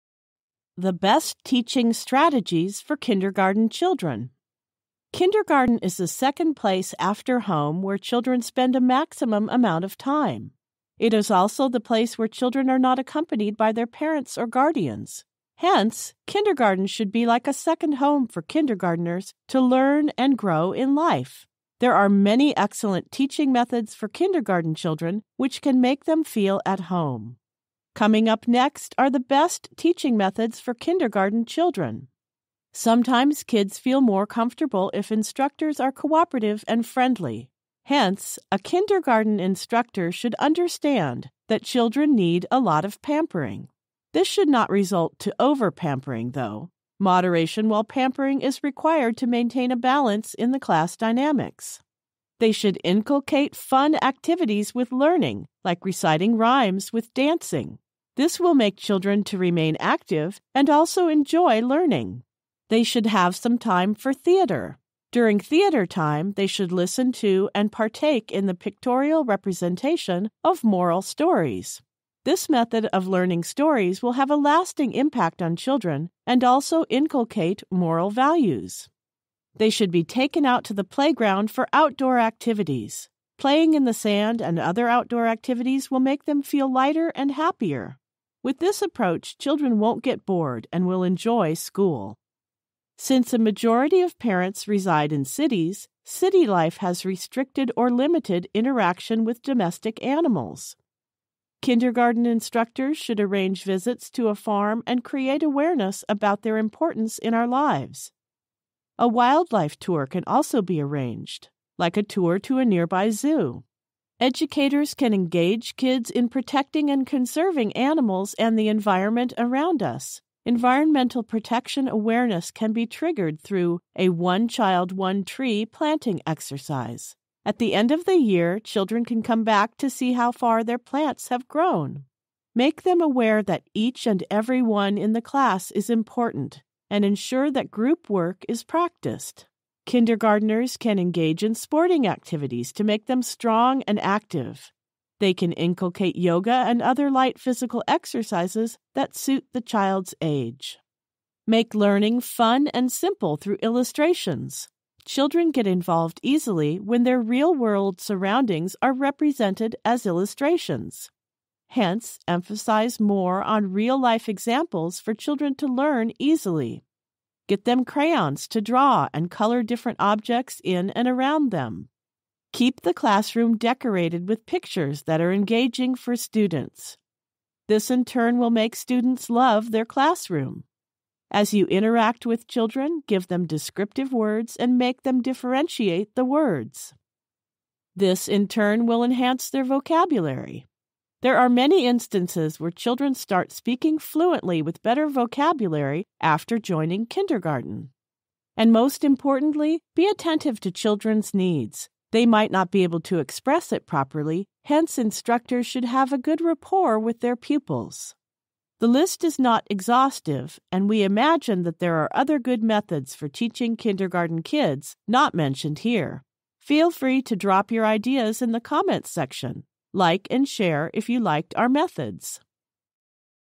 10 teaching strategies for kindergarten - Ten Tips For Teaching Kindergarten Children
This video focuses on 10 teaching strategies for kindergarten - Ten Tips For Teaching Kindergarten Children. Teaching children at a young age can be a bit of a challenge especially for new teachers. Understanding some strategies to go about it is paramount to the success of any new teacher in the industry. This video equips teachers and parents with new ideas for going over this huddle. These tips that can be applied in the classroom or in outdoor playgrounds. Remember to share with us any feedback on the subject.